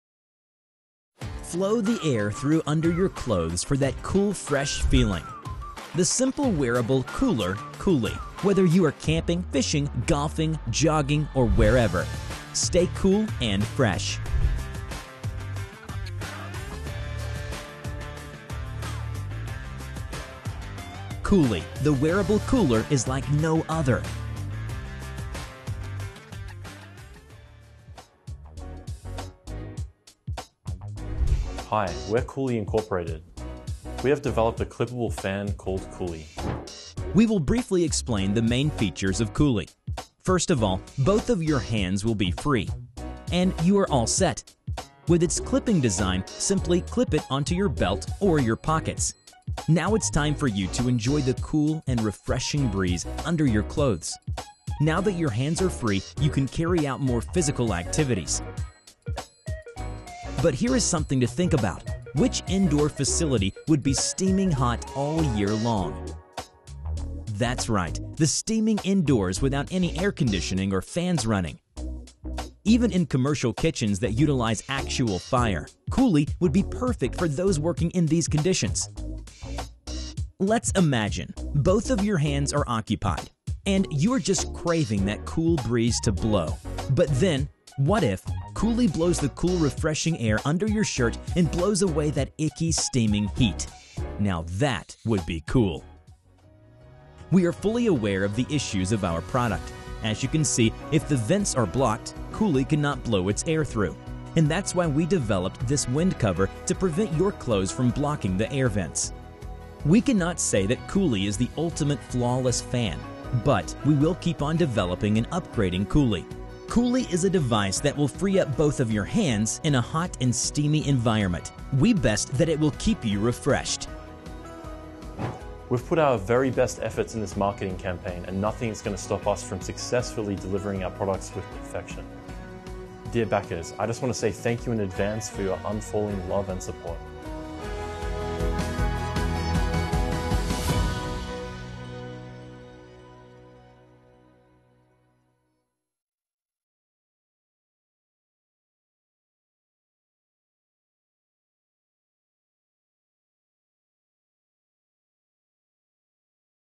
COOLY : THE HANDS-FREE PORTABLE WEARABLE CLIP FAN | Kickstarter | Gizmo Hub
Clip-Up and Free-Up Both Hands! Breezing Air Between Your Clothes. Heat Evaporation = Instant Cooling